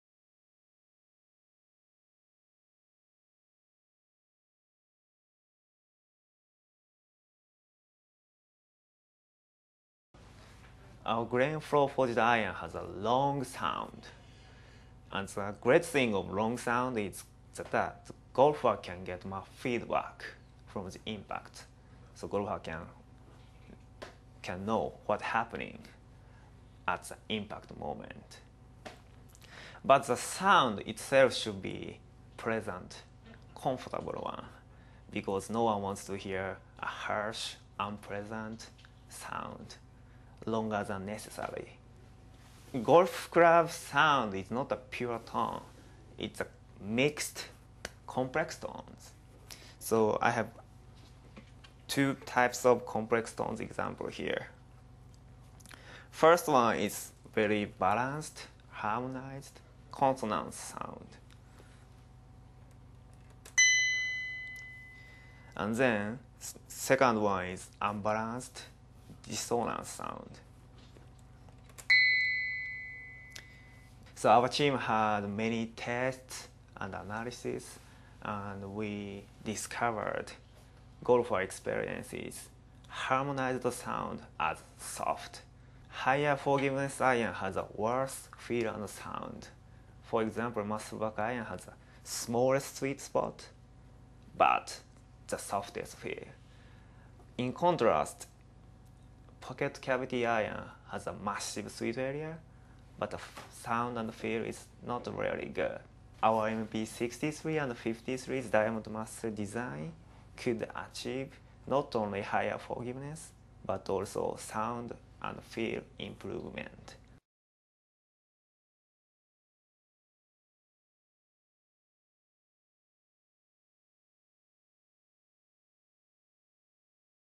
Harmonic Impact Technology
Mizuno R&D examine the role of sound in 'feel'.